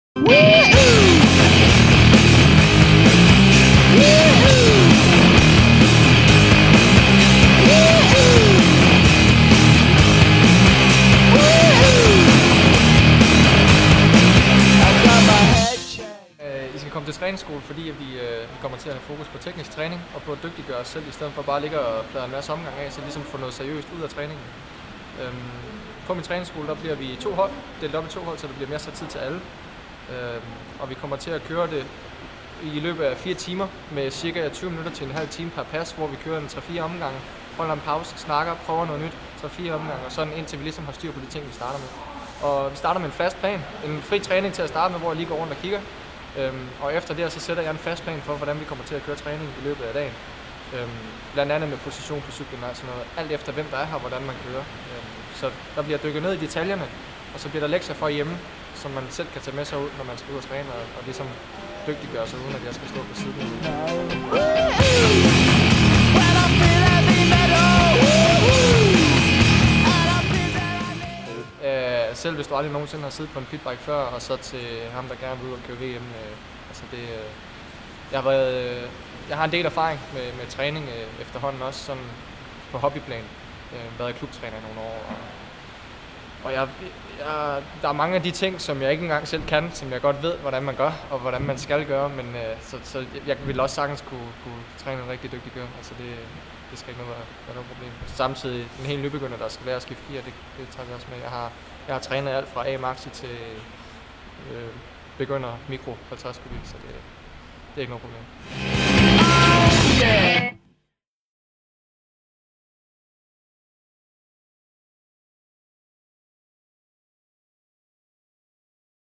Pitbike Trænings skole
Træning skole for Pitbike, arrengeret af Sebastian Kærsgaard og Lars Lehmkuhl som bliver afholdt på Tørring indoor Mx bane på følgene datoer:

4.Dec
18.Dec
15.Jan 17

Flere oplysninger om træningsskolen kan findes på:
Facebook.dk/pitstardk/events